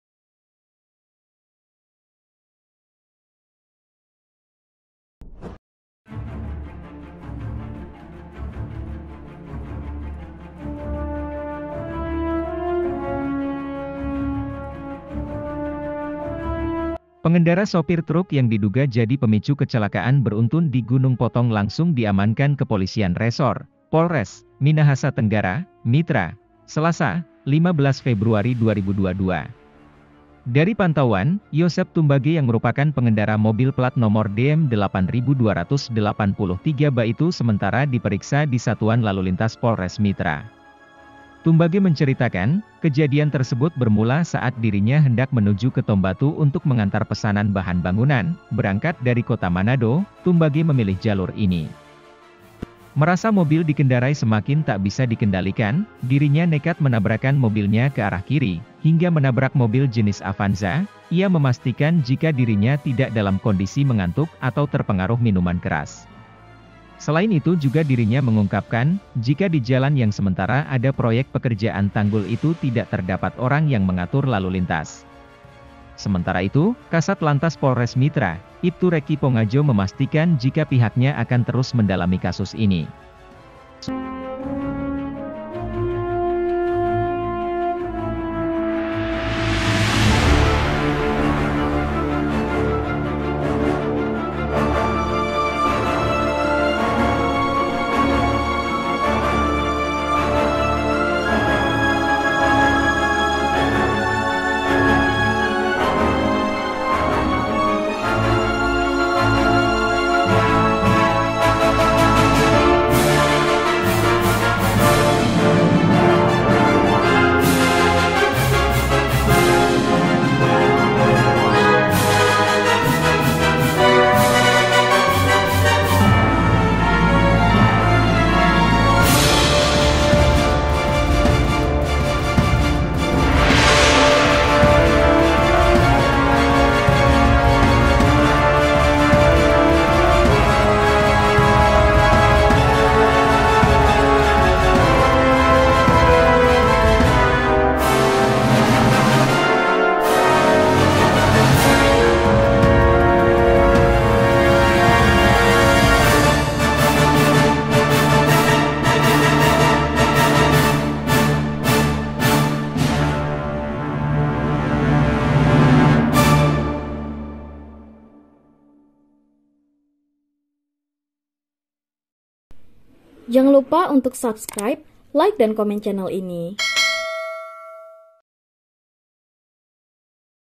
Pengakuan Sopir Truk Penyebab Kecelakaan Beruntun di Minahasa Tenggara
POSBELITUNG.CO - Sopir truk yang diduga jadi pemicu kecelakaan beruntun di Gunung Potong langsung diamankan Kepolisian Resor (Polres) Minahasa Tenggara (Mitra), Selasa (15/2/2022).
Dari pantauan, Yosep Tumbage yang merupakan pengendara mobil plat nomor DM 8283 BA itu sementara diperiksa di Satuan Lalulintas Polres Mitra.
Tumbage menceritakan, kejadian tersebut bermula saat dirinya hendak menuju ke Tombatu untuk mengantar pesanan bahan bangunan.
Berangkat dari Kota Manado, Tumbage memilih jalur ini.
"Awalnya rem bisa digunakan, hanya saja pas sudah dekat tiba-tiba rem blong," ucapnya.
Menurutnya, saat kejadian itu dirinya tetap tenang dan berusaha memilih jalur terbaik. Ini dilakukan untuk menghindari potensi terburuk.
"Kita langsung pilih jalur kiri, karena kita liat di situ paling aman. Soalnya, kalau dipaksakan jalur kanan, besar kemungkinan akan ada korban karena dari arah berlawanan ada juga kendaraan," terangnya.
Merasa mobil yang dikendarai semakin tak bisa dikendalikan, dirinya nekat menabrakan mobilnya ke arah kiri, hingga menabrak kendaraan mobil jenis Avanza.
Ia memastikan jika dirinya tidak dalam kondisi mengantuk atau terpengaruh minuman keras.
"Kita so beberapa kali lewat sini, tapi baru kali ini kena musibah. Kalau mengantuk dan mabuj yang pasti tidak," ujarnya.
Selain itu juga dirinya mengungkapkan, jika di jalan yang sementara ada proyek pekerjaan tanggul itu tidak terdapat orang yang mengatur lalu lintas.
"Waktu kejadian sih tidak ada orang," tandasnya sembari menambahkan dirinya menyerahkan semua proses ke pihak berwajib.
"Semua terjadi sangat cepat. Tidak ada yang mau celaka begini, karena sudah terjadi kami serahkan kepada pihak berwajib," pungkasnya.
Sementara itu, Kasat Lantas Polres Mitra, Iptu Recky Pongajow memastikan jika pihaknya akan terus mendalami kasus ini.
"Kami akan lakukan pemeriksaan terlebih dahulu. Kami akan cari tau semua pemicu kecelakaan," kuncinya.(*)

Editor: Hery